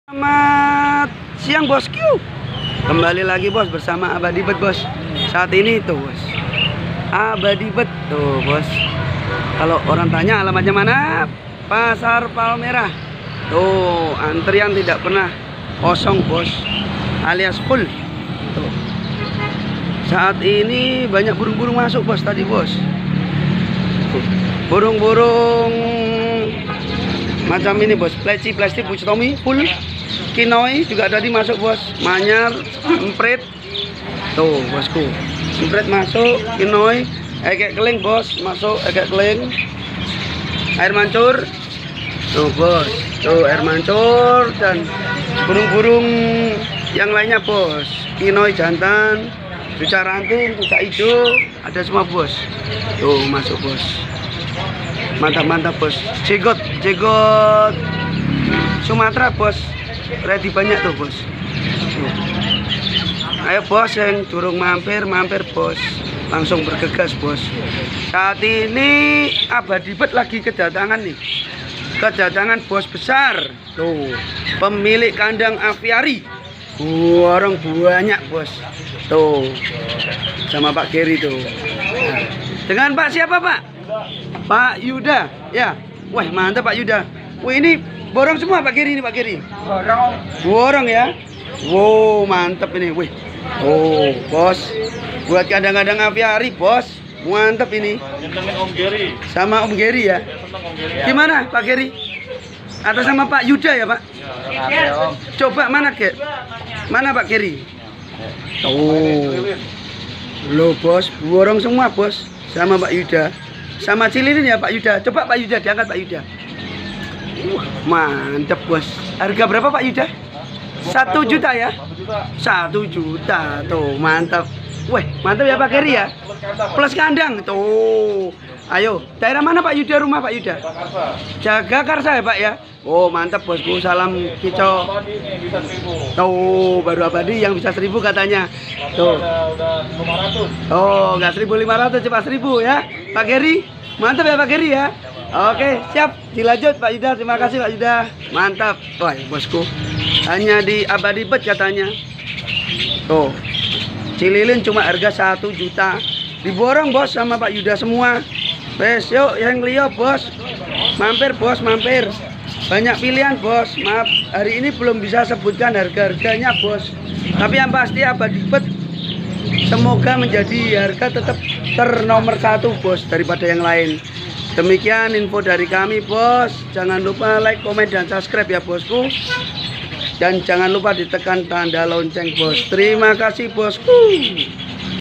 Masuk burung sumatraan stok melimpah bos + kedatangan bos besar borong burung untuk aviarynya.
Bisa melalui toko online kami atau kirim manual